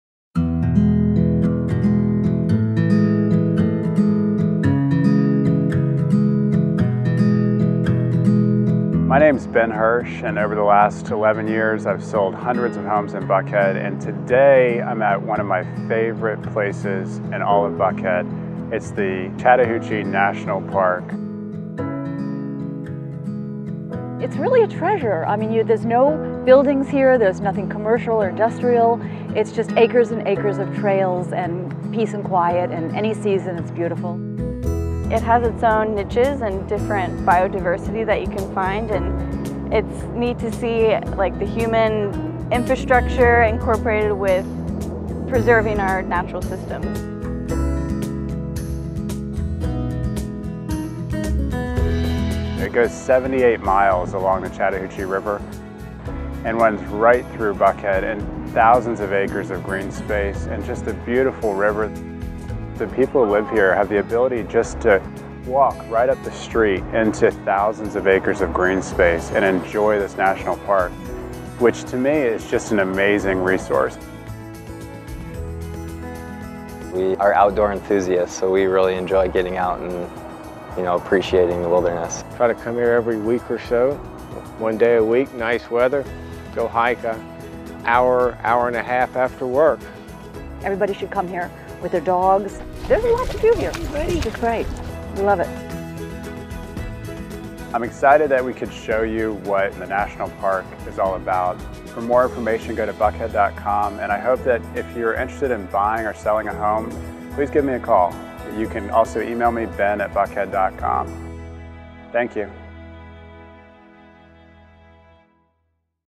Chattahoochee River National Park
Buckhead’s Chattahoochee National Park is a sanctuary in the city. One of the allures of Buckhead living is the juxtaposition of big-city conveniences with the simple pleasures of small rural communities.